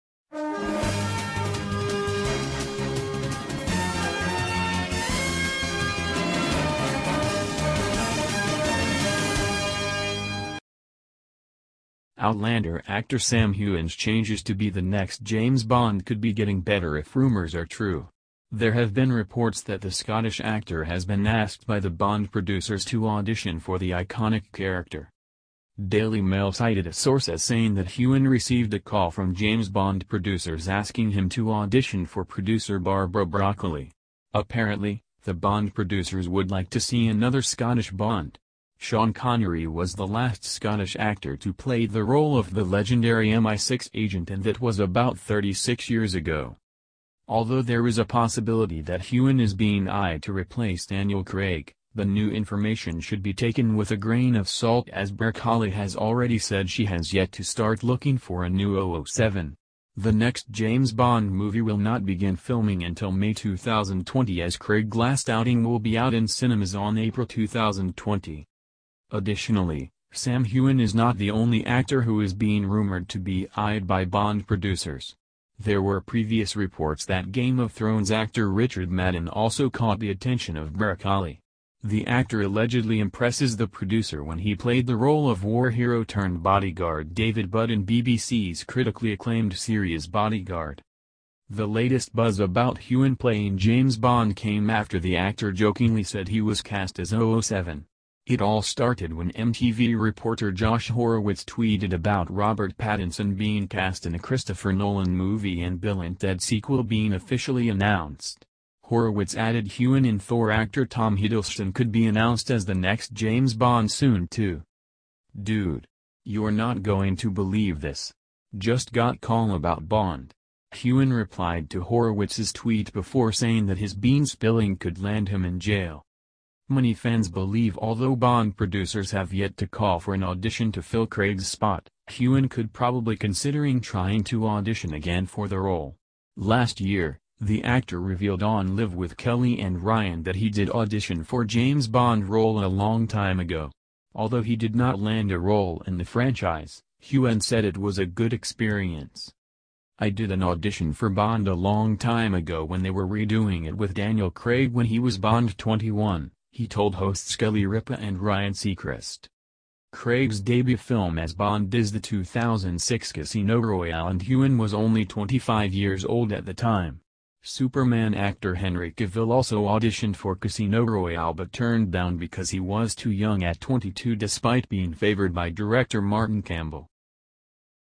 'Outlander' Actor Sam Heughan secretly holds a wedding with Caitriona Balfe
'Outlander' Actor Sam Heughan Rumored To Have Been Asked By James Bond Bosses To Audition As Daniel Craig's Replacement